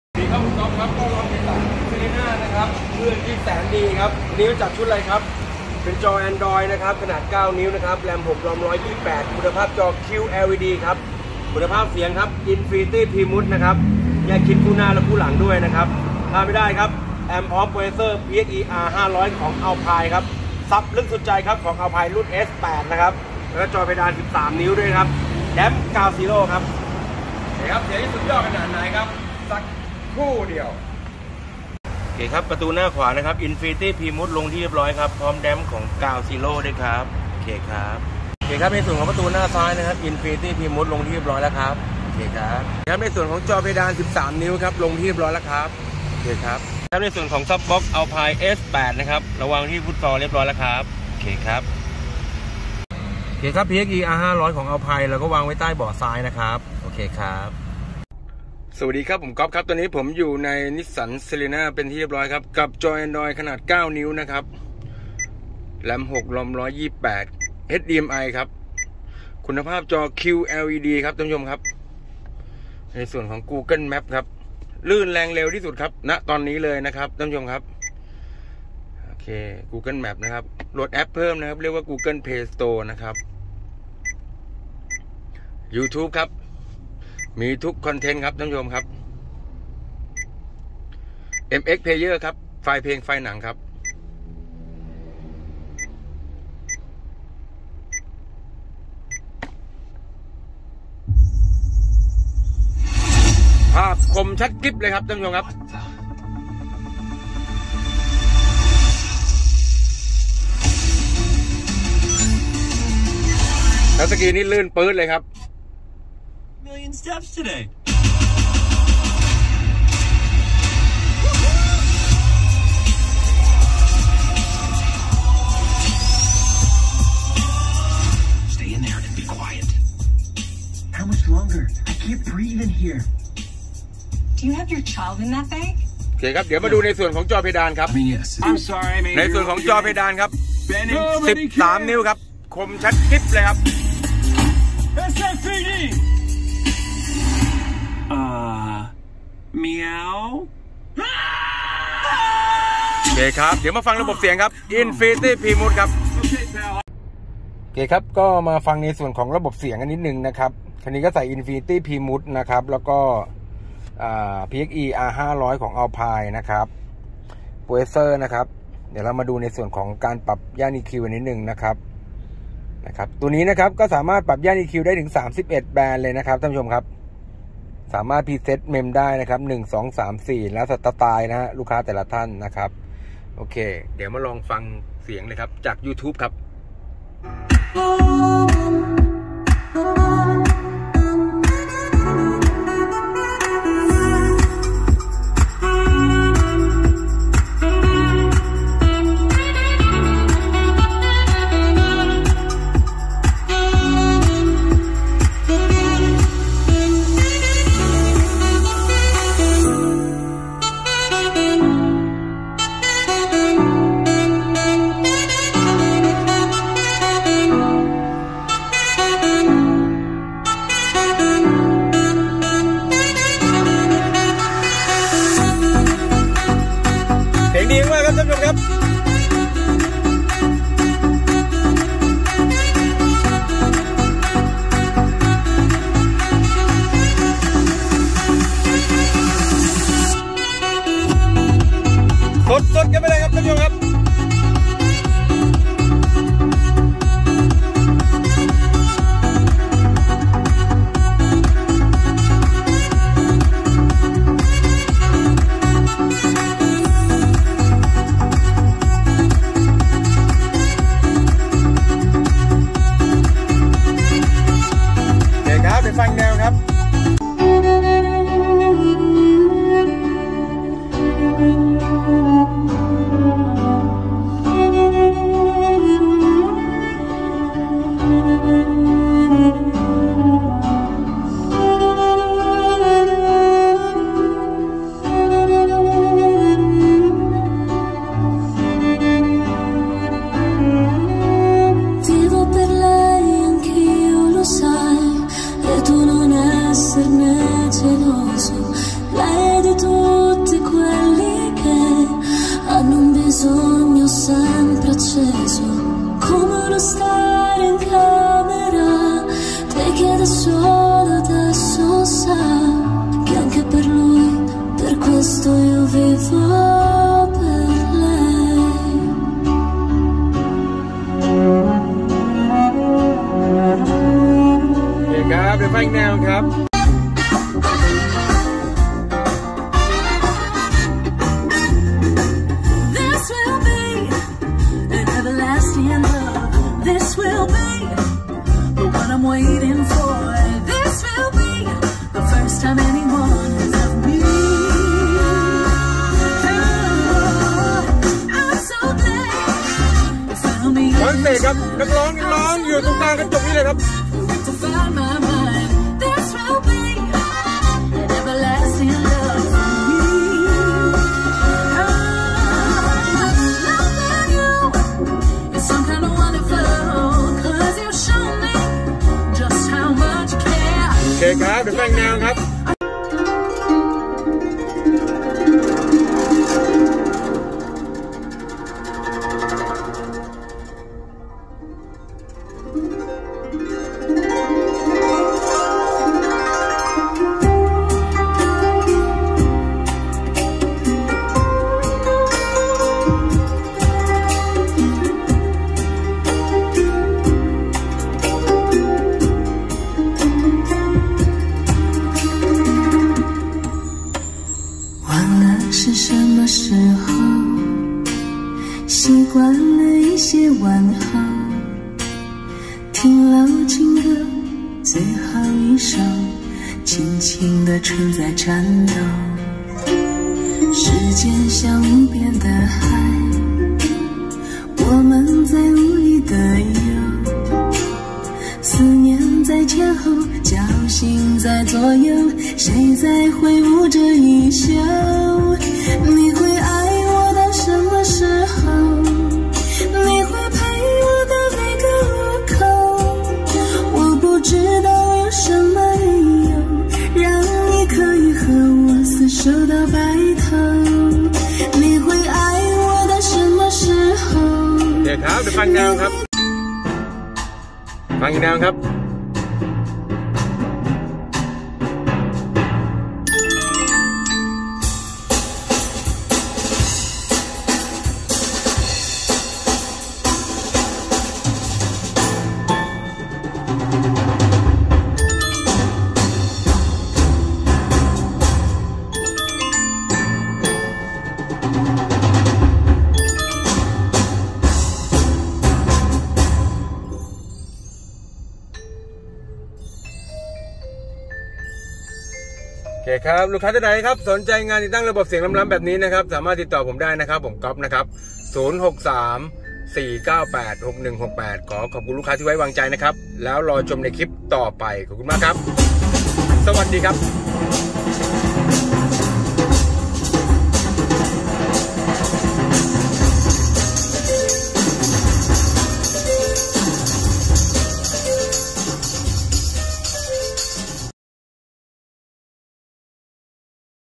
Nissan Serena จอ android 9นิ้ว QLED +Infinity +Alpine R500 +Sub AlpineS8 063-4986168
ต้อนรับ Nissan Serena
กับชุด INFINITY BY HARMAN
alpine PXE-R500
Sub box Alpine S8
Damp Ground zero
จอเพดาน 13นิ้ว